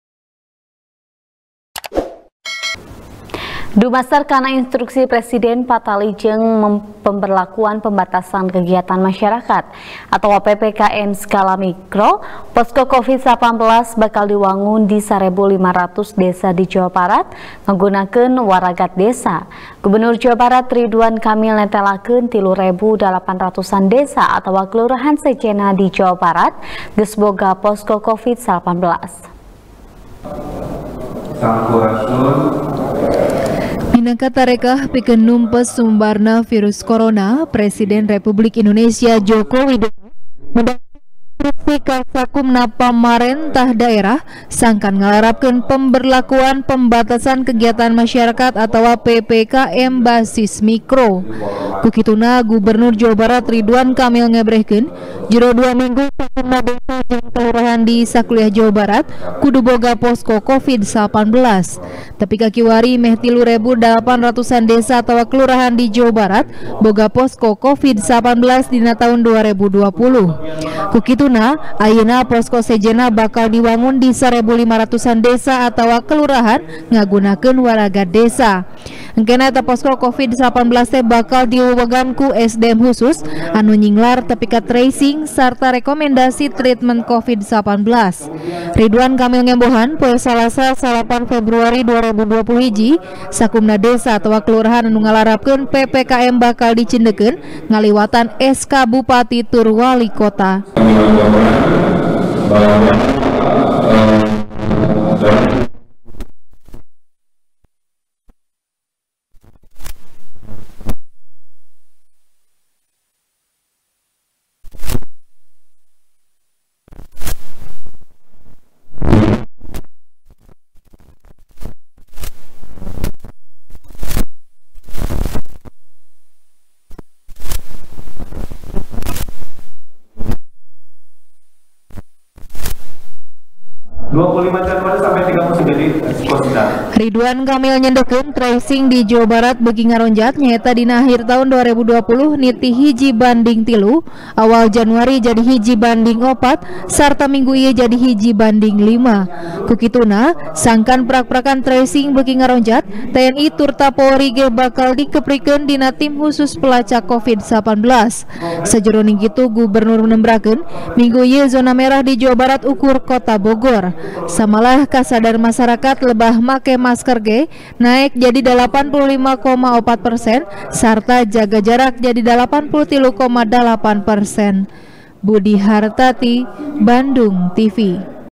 Posko Covid-19 Bakal Ngadeg Di 1.500-An Desa | TP | BANDUNGTV
TP | BANDUNGTV | ASTRI CAHYANI | Dumasar Kana Instruksi Presiden Patali Jeung Pemberlakukan Pembatasan Kegiatan Masyarakat Atawa PPKM Skala Mikro, Posko Covid19 Bakal Diwangun Di Sarebu Lima Ratusan Desa Di Jawa Barat, Ngagunakeun Waragad Desa. Gubernur Jawa Barat Ridwan Kamil Netelakeun, 3 Rebu 8 Ratusan Desa Atawa Kelurahan Sejenna Di Jawa Barat Geus Boga Posko Covid19.